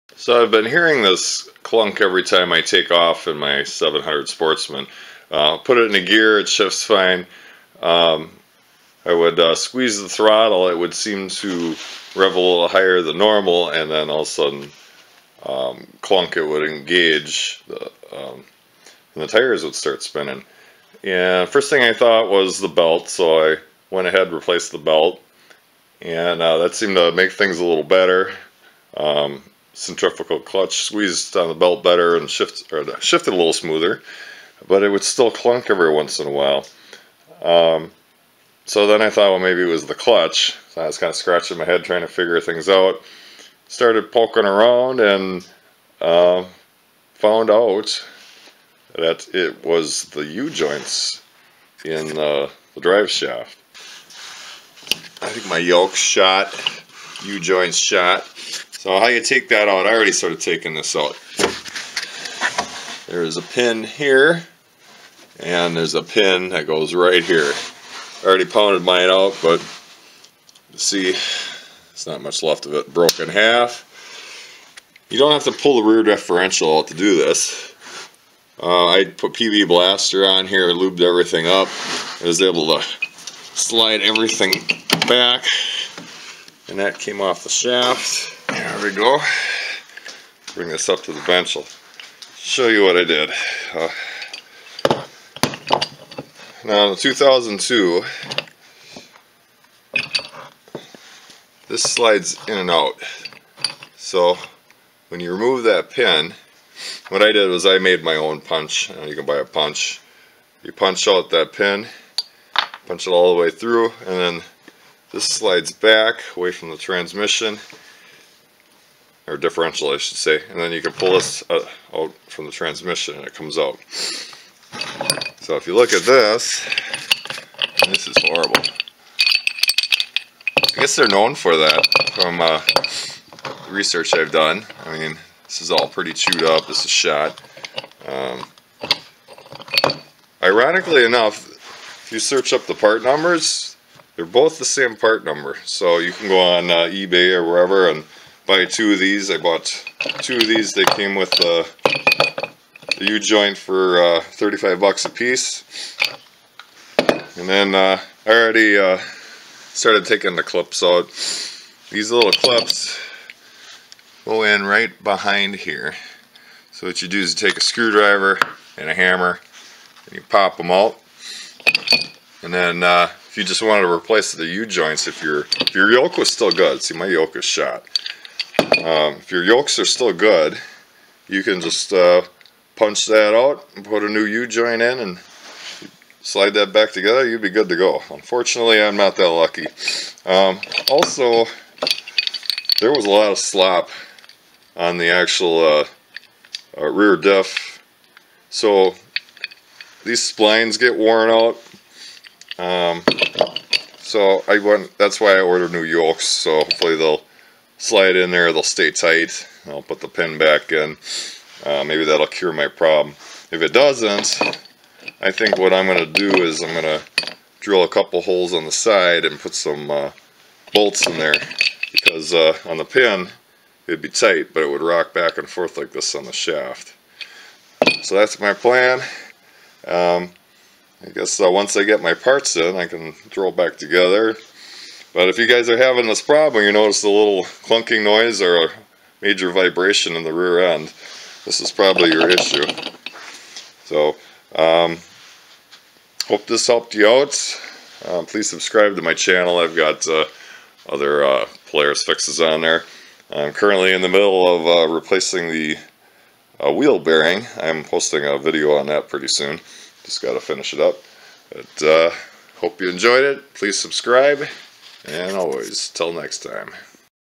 Polaris Sportsman Clunks at take off! Prop Shaft (Rear Axle)Removal
Clunking or vibrating when accelerating? I'll show you why and how to remove the Prop Shaft.